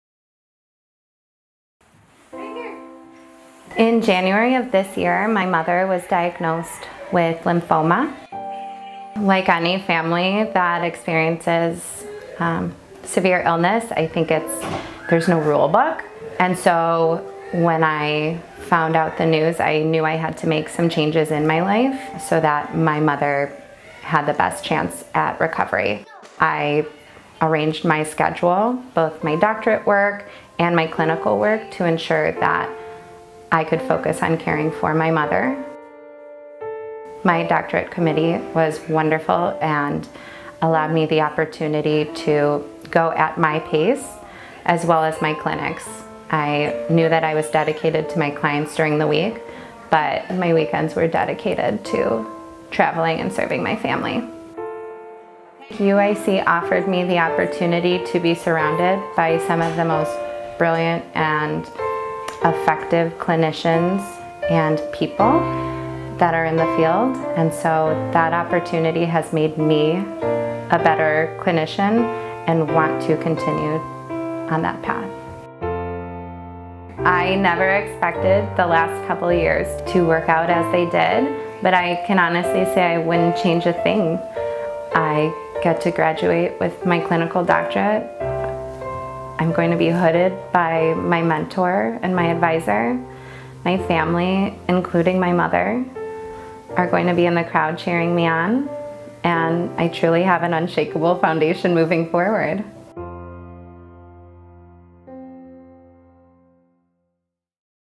Inspiring Grad: Andrea Gurga, Occupational Therapy '19
Gurga balanced a serious family illness while pursuing her doctorate.